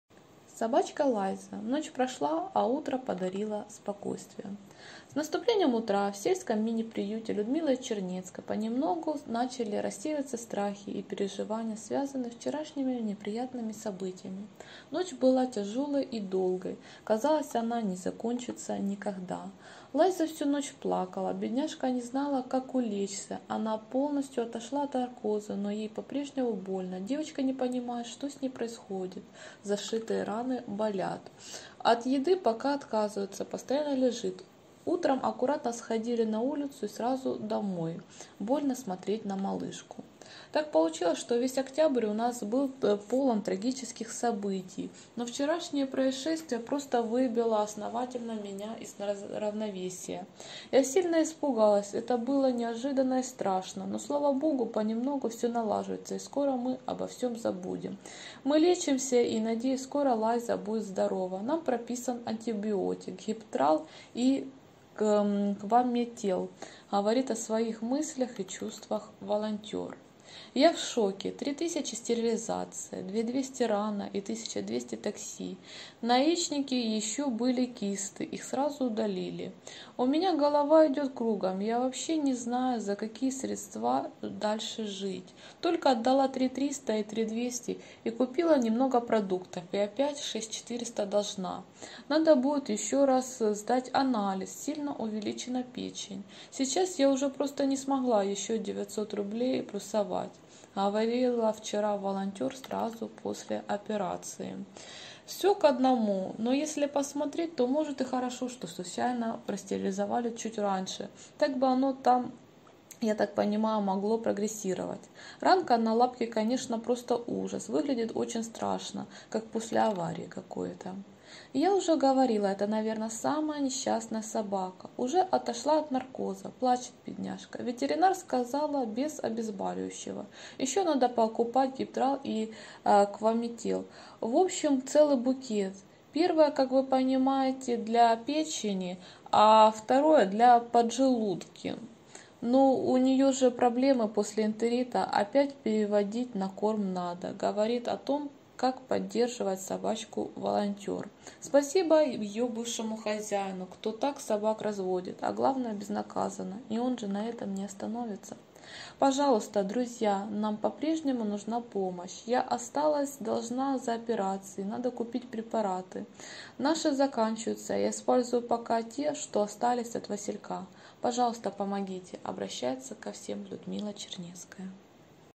Наша Лайза: теперь мы спокойные и аккуратные, как никогда!)
Материал для ролика взят из статьи: "Собачка Лайза: ночь прошла, а утро подарило спокойствие"